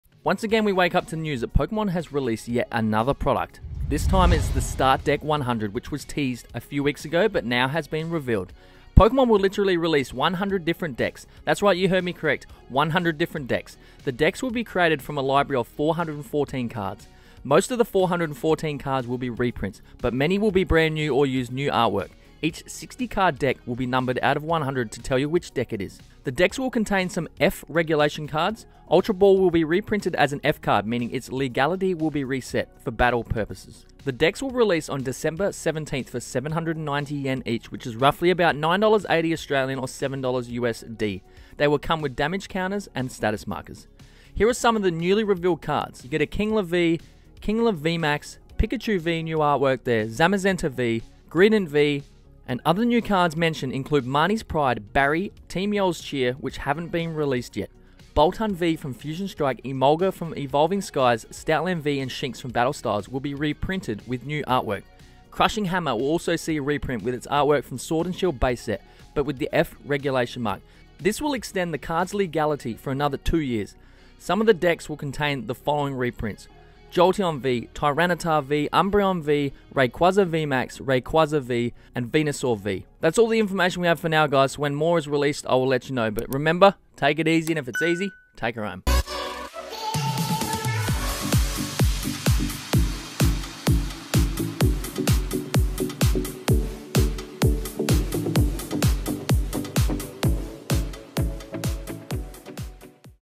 Start Deck 100 - NEW Pokemon Japanese Product!
The “Start Deck 100” we teased a few weeks ago has now been revealed!

Pokemon will literally release 100 different decks! The decks will be created from a library of 414 cards. Most of the 414 cards will be reprints, but many will be brand new or use new artwork. Each 60-card deck will be numbered out of 100 to tell you which deck it is.

West Footscray, Victoria
Australia 3012

Remember take it easy and it it's easy, take it home!
*All prices in this video are averages of current sold listings on Ebay Aus*